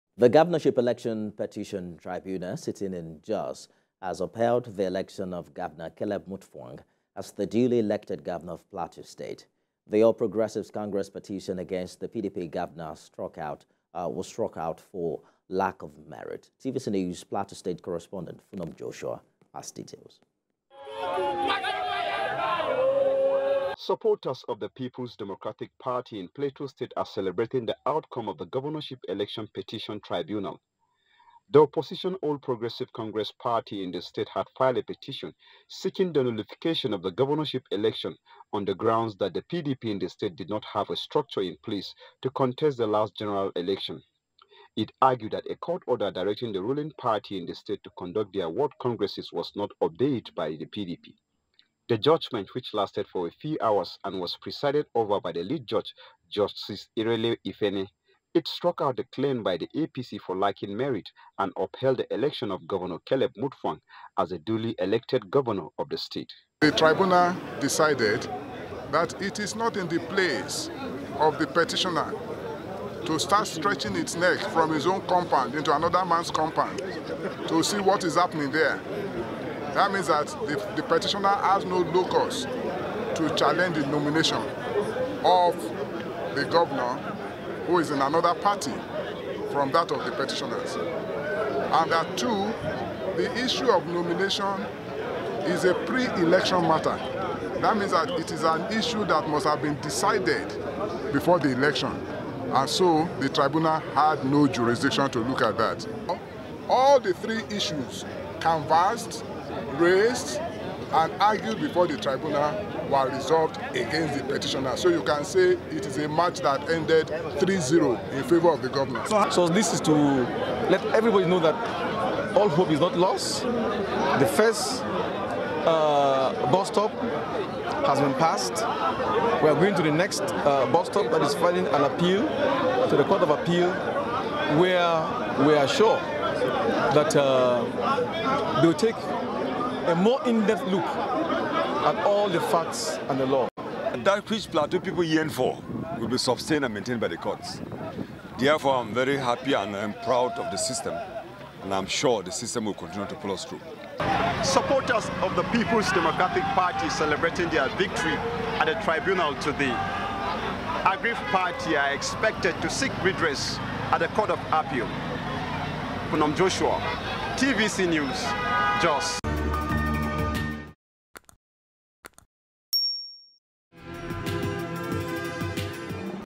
Tribunal Affirms Caleb Muftwang as Duly Elected Governor
Governorship Election Tribunal sitting in Jos has upheld the election of Governor Caleb Mutfwang as the duly elected Governor of Plateau State.

The All Progressives Congress petition against the PDP Governor struck out for lack of merit.